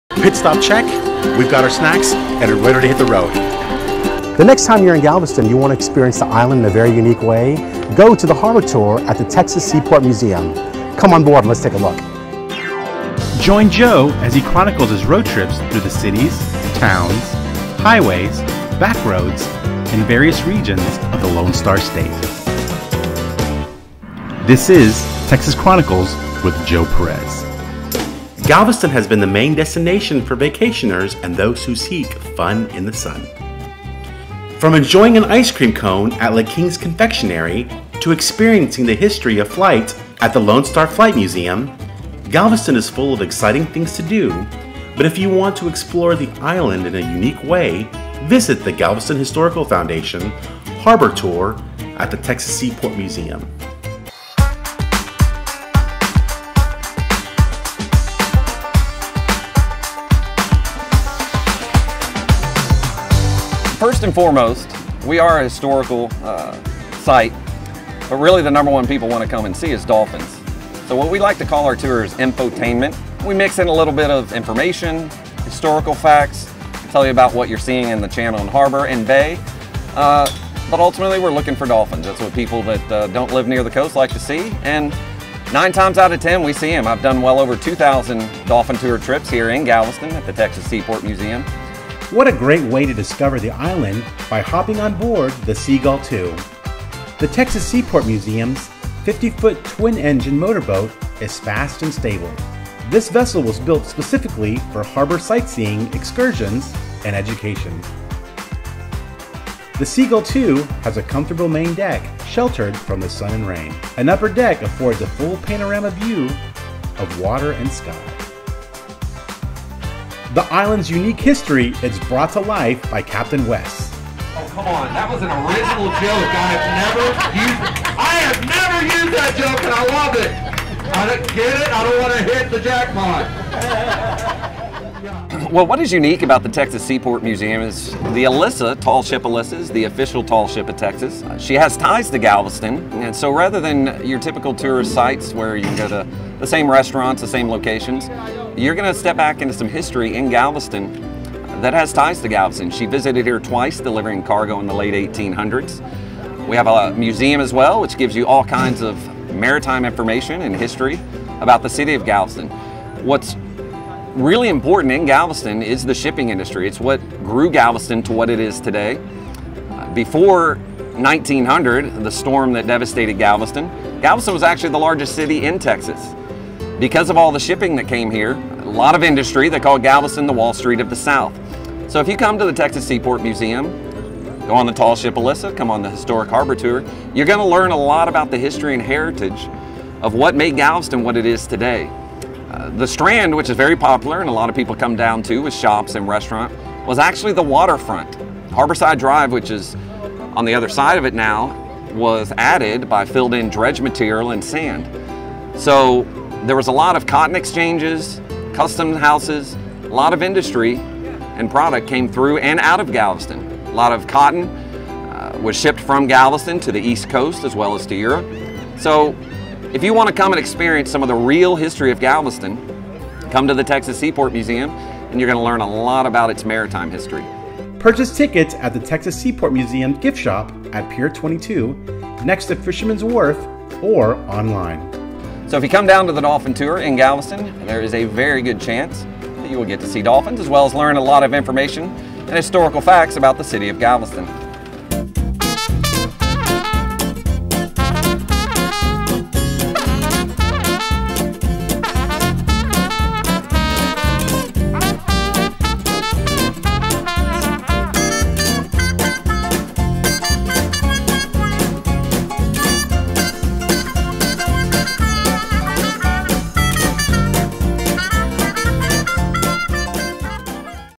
Texas Chronicles: Come on Board the Seagull II
My adventure takes me to Galveston, Texas!
I had the opportunity to hop on board the Seagull II for some dolphin watching and discover the island in a new and unique way.

The Texas Seaport Museum's, Galveston Harbor Tour offers an interesting and history driven view of the island while spotting dolphins as they peek up around the boat. The Seagull II is a 50-foot twin-engine vessel that was built specifically for harbor sightseeing excursions and education.